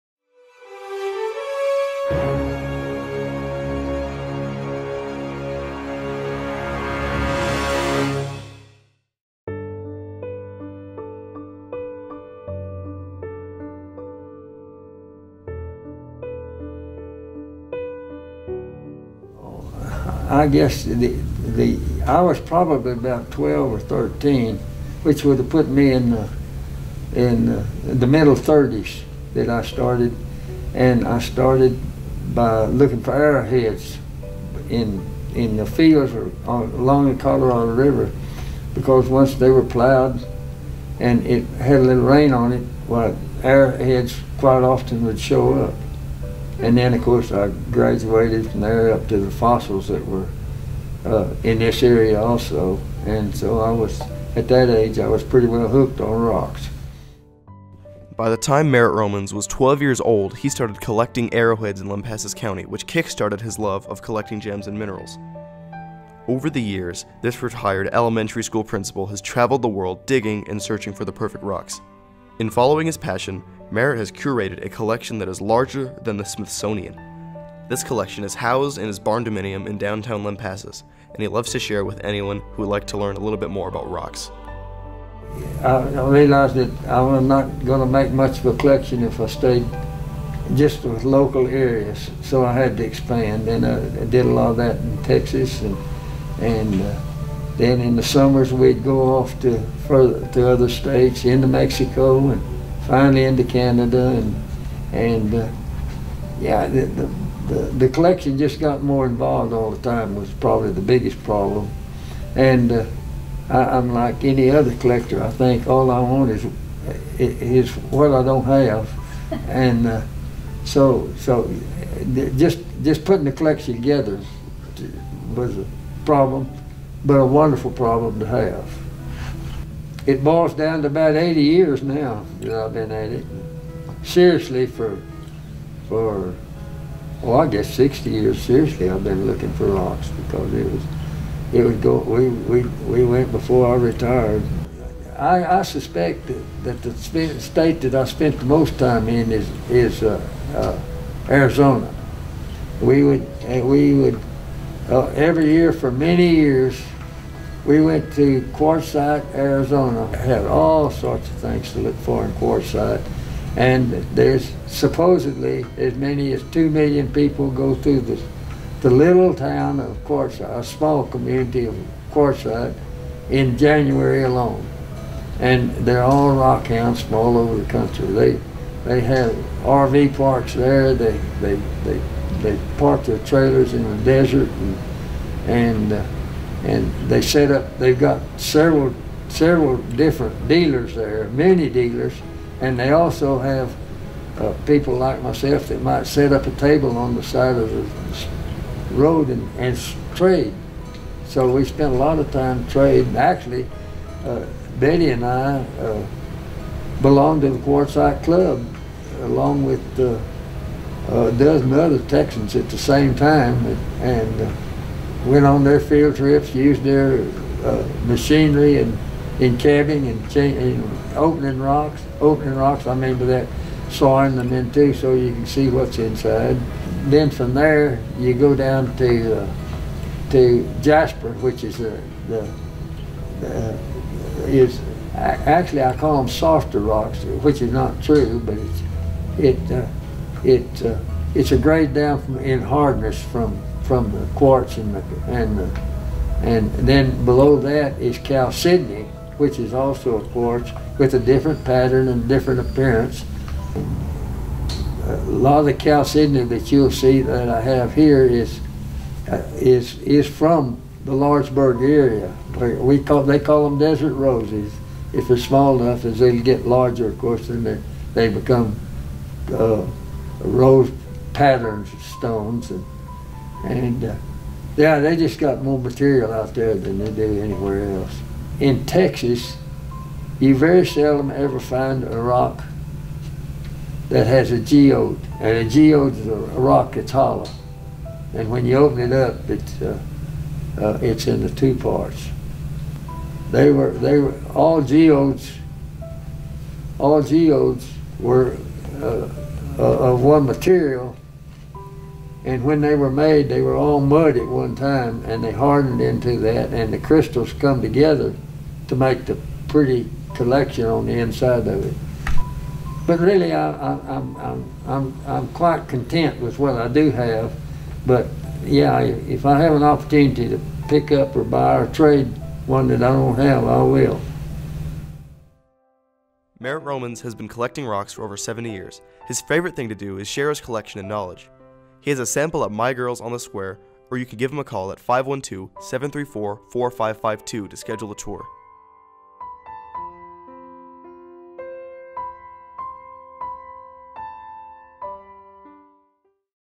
Hidden Gem
By the time Merritt Romans was 12 years old he started collecting arrowheads in Lampasas county, which kick-started his love of collecting gems and minerals. Over the years this retired elementary school principal has traveled the world digging and searching for the perfect rocks. Pursuing his passion, Merritt has curated a collection that is larger than the Smithsonian. This collection is housed in his barndominium in downtown Lampasas and he loves to share it with anyone who would like to learn a little more about rocks.